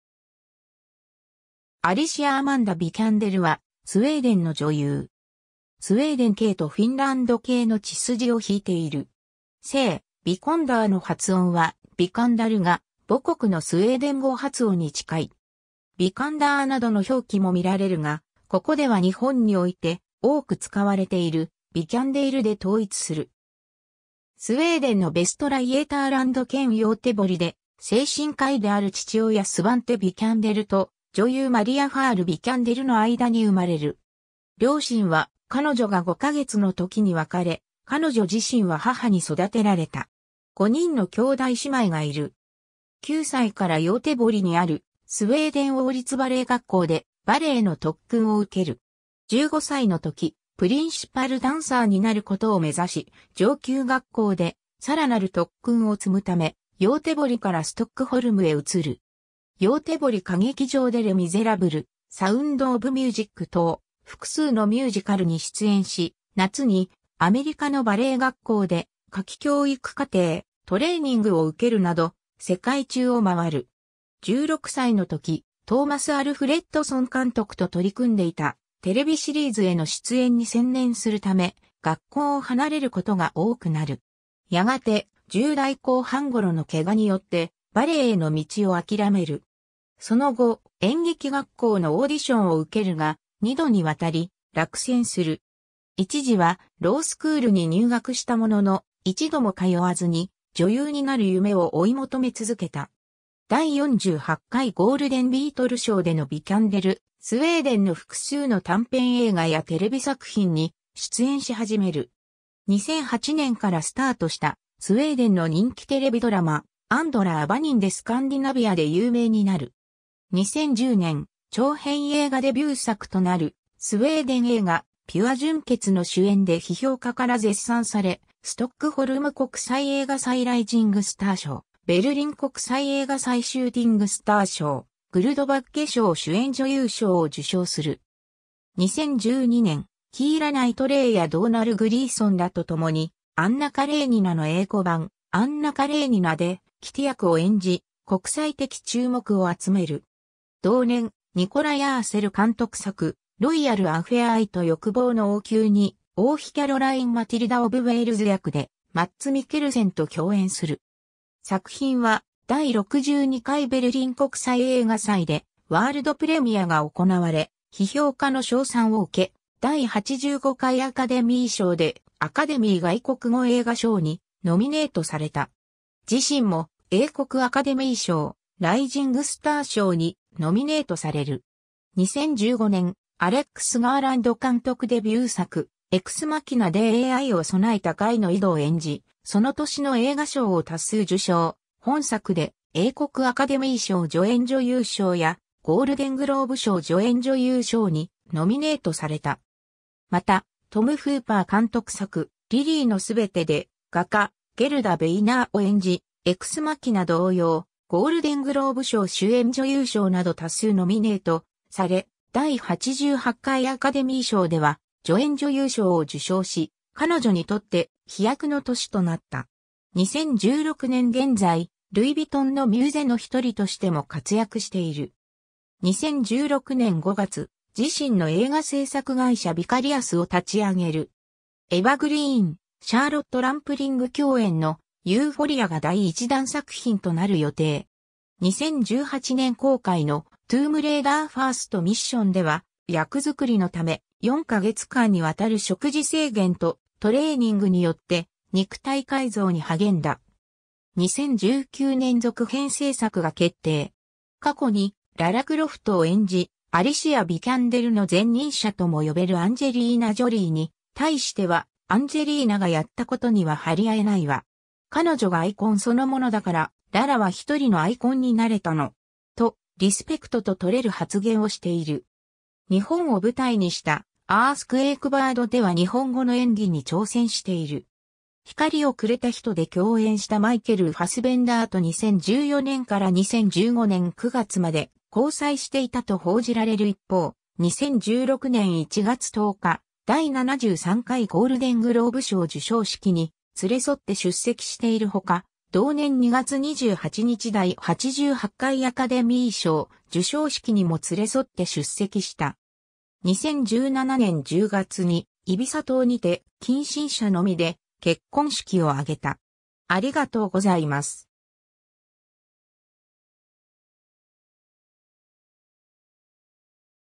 アリシア・ヴィキャンデル
アリシア・アマンダ・ヴィキャンデル（Alicia Amanda Vikanderスウェーデン語発音: [aˈliːsɪa aˈmanda vɪˈkandɛr];1988年10月3日 - ）は、スウェーデンの女優。
スウェーデン系とフィンランド系の血筋を引いている。
姓「Vikander」の発音は「ヴィカンダル」が母国のスウェーデン語発音に近い。
「ヴィカンダー」などの表記も見られるが、ここでは日本において多く使われている「ヴィキャンデル」で統一する。
 スウェーデンのヴェストラ・イェーターランド県ヨーテボリで、精神科医である父親スヴァンテ・ヴィキャンデルと女優マリア・ファール・ヴィキャンデル（Maria Fahl Vikander）の間に生まれる。
両親は彼女が5か月の時に別れ、彼女自身は母に育てられた。
 5人の兄弟姉妹がいる。
 9歳からヨーテボリにあるスウェーデン王立バレエ学校（Kungliga Svenska Balettskolan）でバレエの特訓を受ける。
 15歳の時、プリンシパルダンサーになることを目指し、上級学校でさらなる特訓を積むため、ヨーテボリからストックホルムへ移る。
ヨーテボリ歌劇場でレ・ミゼラブル、サウンド・オブ・ミュージック等、複数のミュージカルに出演し、夏にアメリカのバレエ学校で夏期教育課程、トレーニングを受けるなど、世界中を回る。
 16歳の時、トーマス・アルフレッドソン監督と取り組んでいたテレビシリーズへの出演に専念するため、学校を離れることが多くなる。
やがて、10代後半頃のケガによってバレエへの道を諦める。
 その後、演劇学校のオーディションを受けるが、2度にわたり落選する。
一時は、ロースクールに入学したものの、一度も通わずに、女優になる夢を追い求め続けた。
 第48回ゴールデン・ビートル賞でのヴィキャンデル（2013年） スウェーデンの複数の短編映画やテレビ作品に出演し始める。
2008年からスタートしたスウェーデンの人気テレビドラマ『Andra Avenyn』でスカンディナヴィアで有名になる。
 2010年、長編映画デビュー作となるスウェーデン映画『ピュア 純潔』の主演で批評家から絶賛され、ストックホルム国際映画祭ライジングスター賞、ベルリン国際映画祭シューティングスター賞、グルドバッゲ賞(スウェーデンのアカデミー賞)主演女優賞を受賞する。
 2012年、キーラ・ナイトレイやドーナル・グリーソンらとともにアンナ・カレーニナの英語版『アンナ・カレーニナ』でキティ役を演じ国際的注目を集める。
同年、ニコライ・アーセル監督作『ロイヤル・アフェア 愛と欲望の王宮』に王妃キャロライン・マティルダ・オブ・ウェールズ役でマッツ・ミケルセンと共演する。
作品は第62回ベルリン国際映画祭でワールドプレミアが行われ、批評家の称賛を受け、第85回アカデミー賞でアカデミー外国語映画賞にノミネートされた。
自身も英国アカデミー賞ライジングスター賞にノミネートされる。
 2015年、アレックス・ガーランド監督デビュー作『エクス・マキナ』でAIを備えたガイノイドを演じ、その年の映画賞を多数受賞、本作で英国アカデミー賞助演女優賞やゴールデングローブ賞助演女優賞にノミネートされた。
 また、トム・フーパー監督作『リリーのすべて』で画家ゲルダ・ヴェイナーを演じ、『エクス・マキナ』同様ゴールデングローブ賞主演女優賞など多数ノミネートされ、第88回アカデミー賞では助演女優賞を受賞し、彼女にとって飛躍の年となった。
 2016年現在、ルイ・ヴィトンのミューゼの1人としても活躍している。
 2016年5月、自身の映画製作会社Vikariousを立ち上げる。
エヴァ・グリーン、シャーロット・ランプリング共演の『Euphoria』が第1弾作品となる予定。
 2018年公開の『トゥームレイダー ファースト・ミッション』では役作りのため、4か月間にわたる食事制限とトレーニングによって肉体改造に励んだ。
2019年続編制作が決定。
過去にララ・クロフトを演じ、アリシア・ヴィキャンデルの前任者とも呼べるアンジェリーナ・ジョリーに対しては「アンジェリーナがやったことには張り合えないわ。彼女がアイコンそのものだから、ララは1人のアイコンになれたの。」とリスペクトととれる発言をしている。
 日本を舞台にした『アースクエイクバード』では日本語の演技に挑戦している。
 『光をくれ...